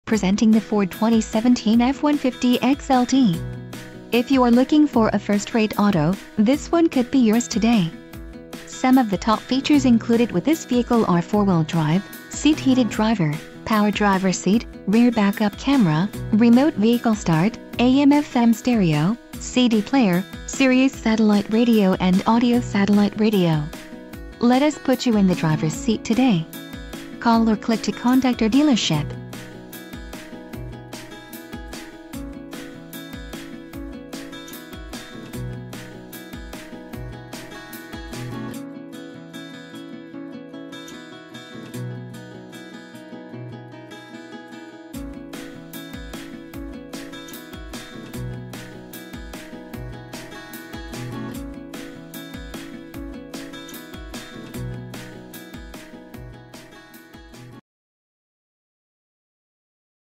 2017 Ford F-150 XLT
Recent Arrival! *LIFETIME WARRANTY INCLUDED!!--CALL FOR DETAILS* Backup Camera, Remote Start, Bluetooth, Complete Detail, Full Tank of Gas, First oil Change free, 4D SuperCrew, 2.7L V6 EcoBoost, 4WD, 10-Way Power Driver's and Passenger Seats, 110V/400W Outlet, 4.2 Productivity Screen in Instrument Cluster, ABS brakes, Auto-Dimming Rear-View Mirror, BoxLink, Chrome Billet Style Grille w/Chrome Surround, Chrome Door and Tailgate Handles w/Body-Color Bezel, Chrome Step Bars, Class IV Trailer Hitch Receiver, Cloth 40/20/40 Front Seat, Compass, Electronic Stability Control, Equipment Group 302A Luxury, Heated Front Seats, Illuminated entry, Leather-Wrapped Steering Wheel, LED Box Lighting, Low tire pressure warning, Power Glass Heated Sideview Mirrors, Power-Adjustable Pedals, Power-Sliding Rear Window, Pro Trailer Backup Assist, Radio: Single-... read more